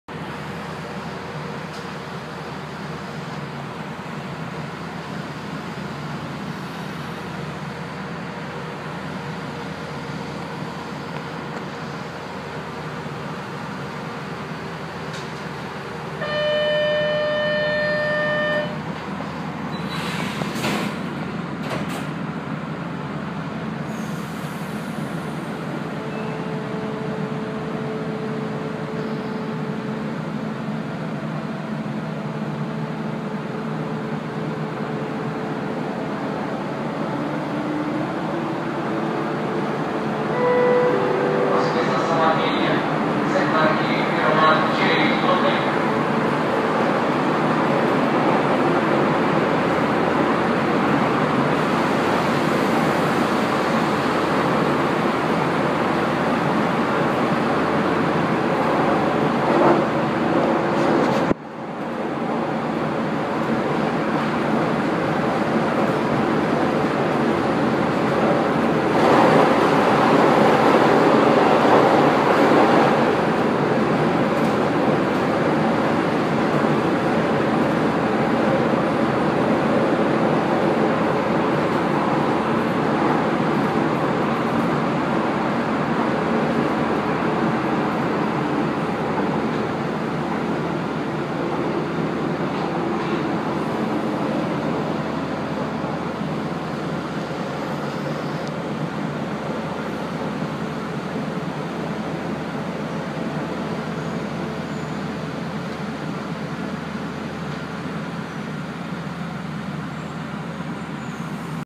Metrô SP: Trecho entre Portuguesa Tietê e Armênia - Linha 1 Azul
Dentro do A21 (Ex A27)
BUDD MAFERSA
Data: 22/11/17

Essa foi a minha penúltima viagem no A21. Esse Budd foi o penúltimo trem da frota pioneira de SP. O A21 foi pra modernização no dia 30/11/2017. Quando ele retornar, estará transformado num Frota I (I21).